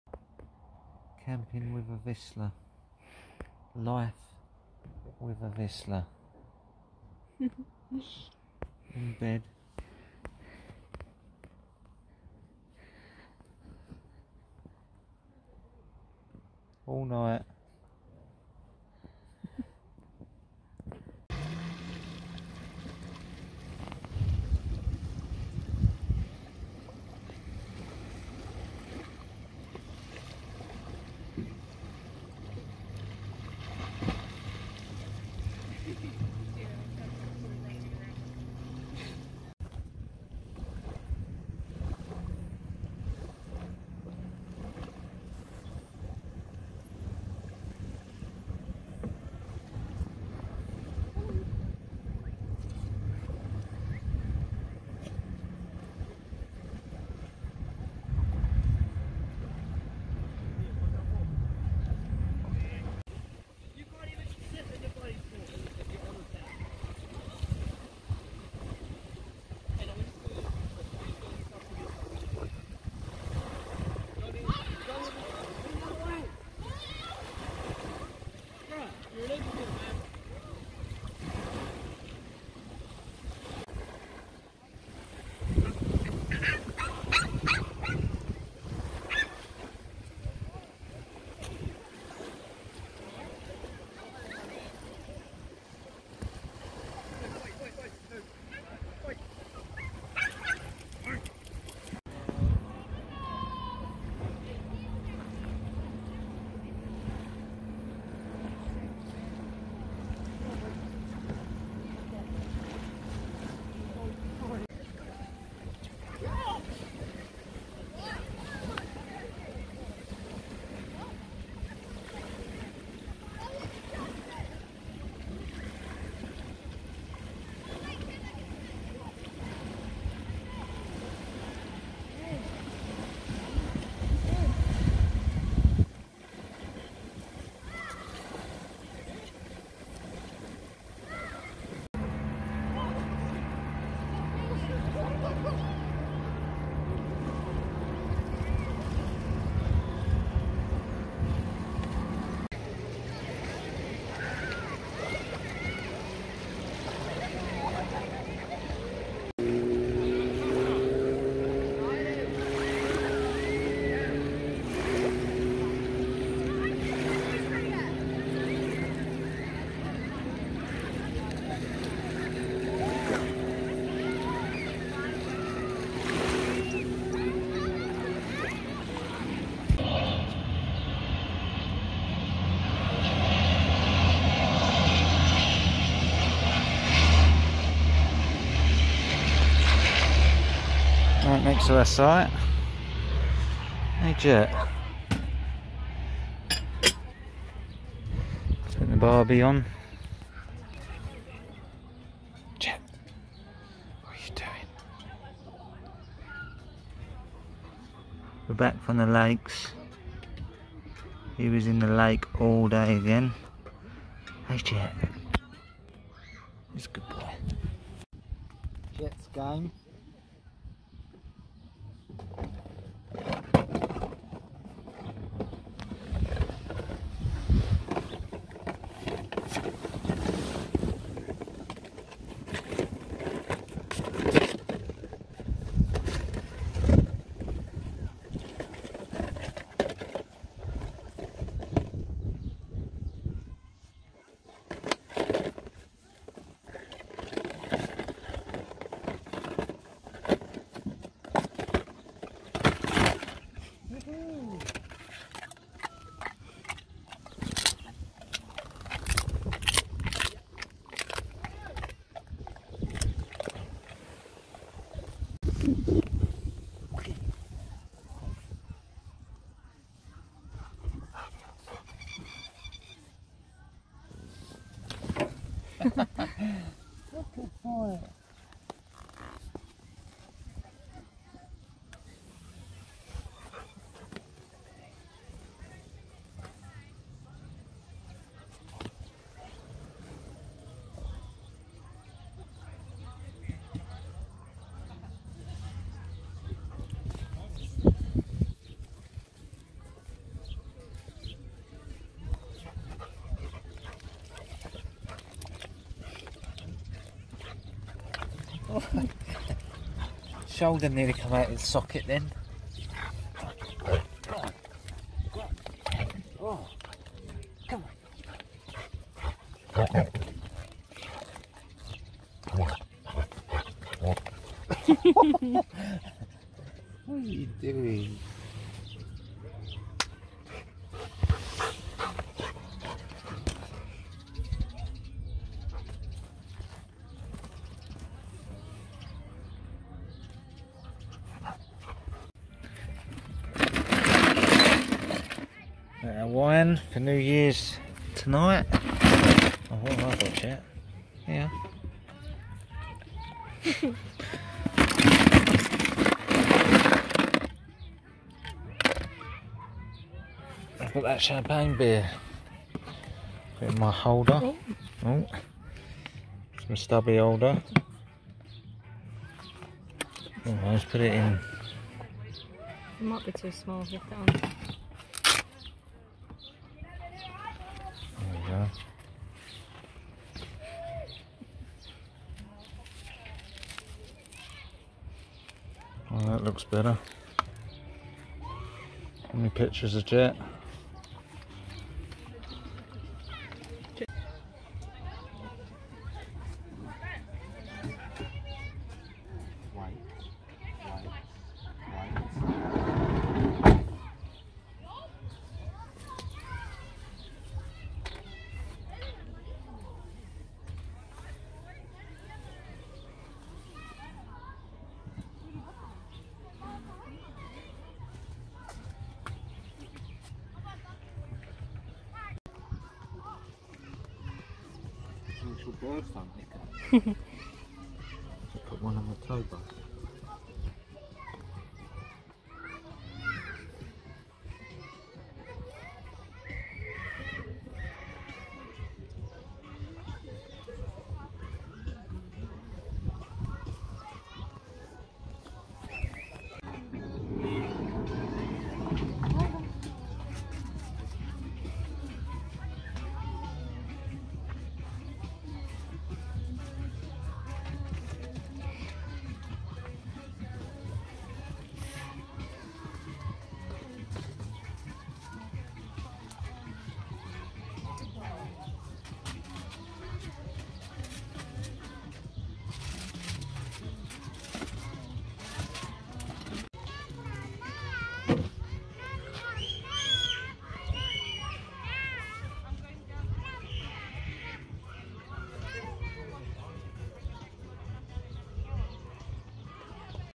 SUP with my Vizsla on the Lake | Holiday Day 2 paddleboarding & camping
Another day at the lake for Jett. He loves the water and enjoys the paddleboard so much!